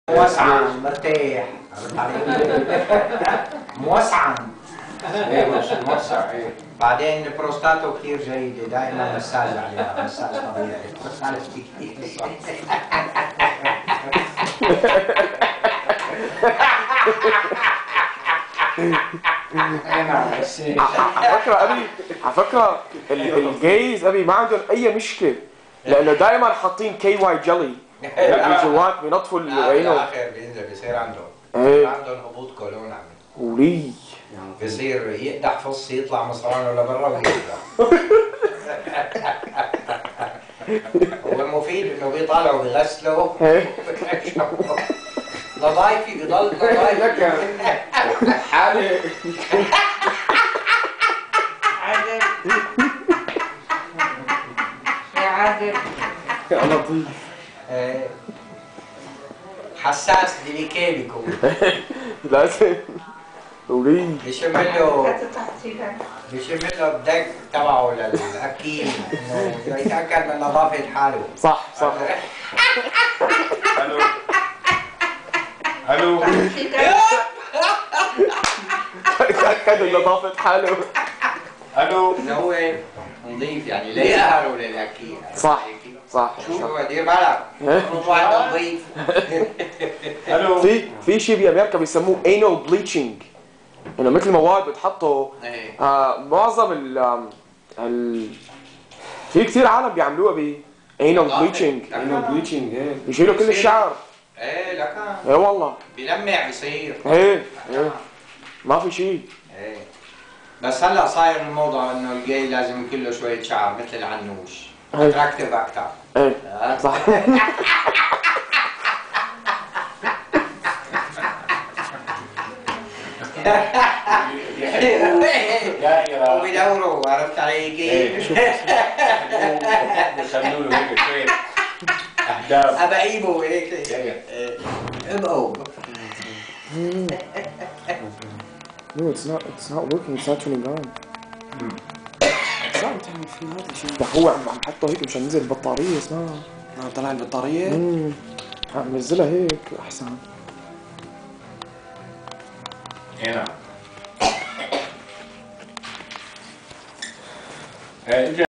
The World Famous Dr. Norman A.K.A Na3man
Take a glimpse of Norman making fun of the gays